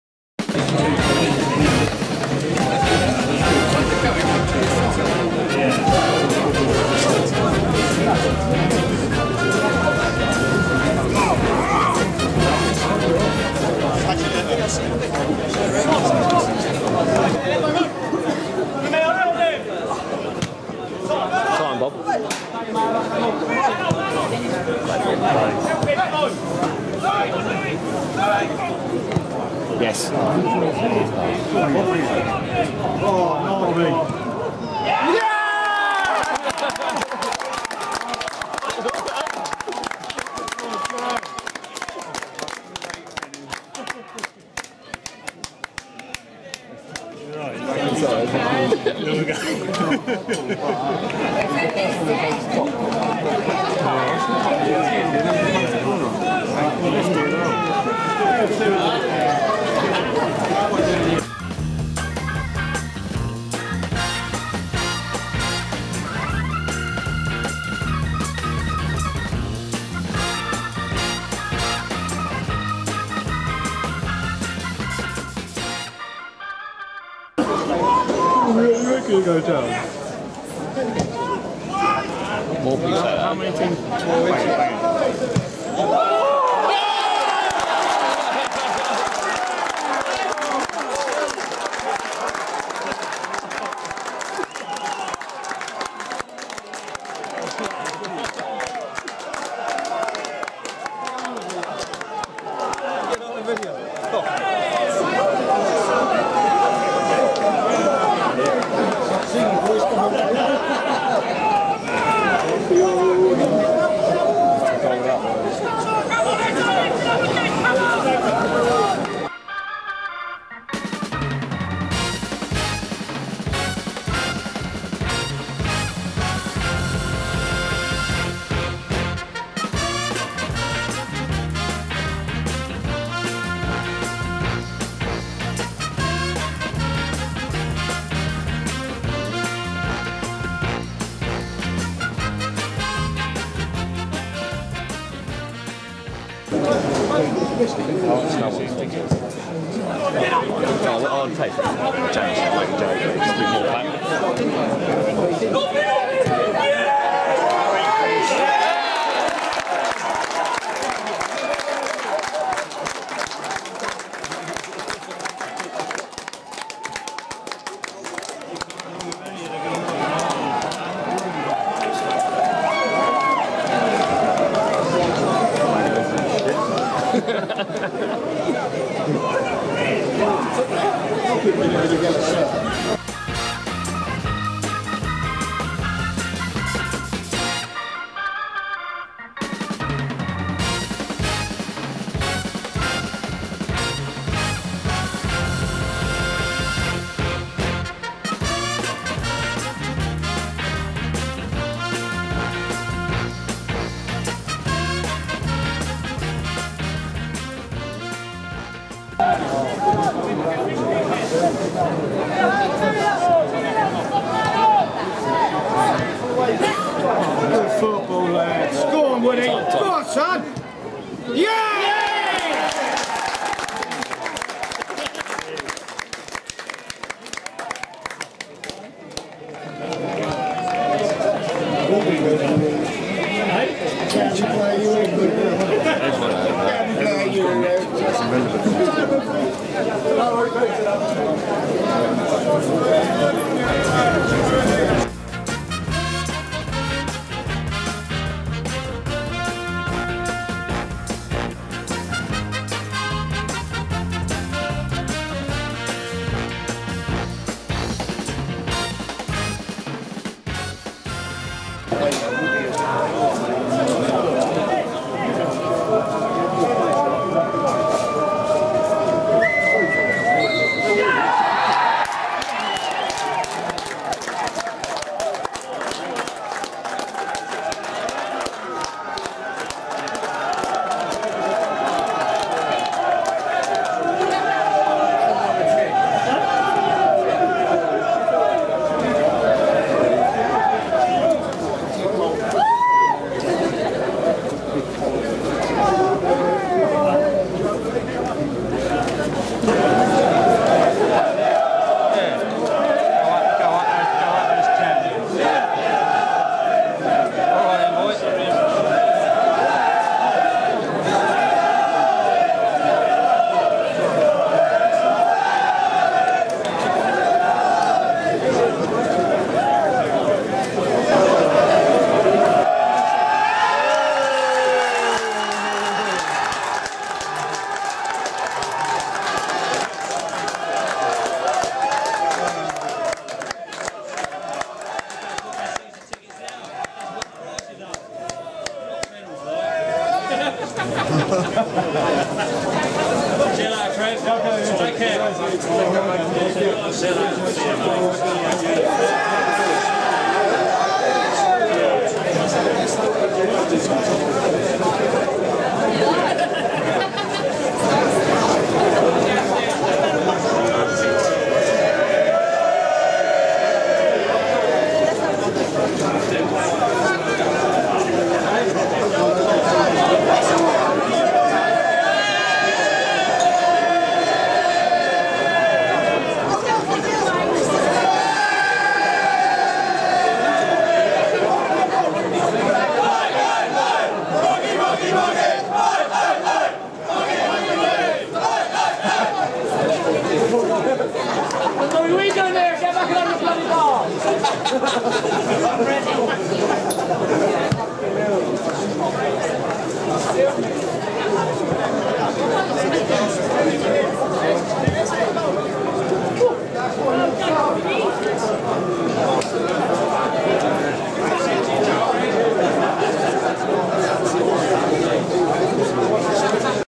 Westfield v Frimley Green - Title Decider
Cherry Red Records Combined Counties Division 1
Final match of the season. Frimley required a draw to take the title, whilst Westfield had to win.
Woking Park, 7th May 2013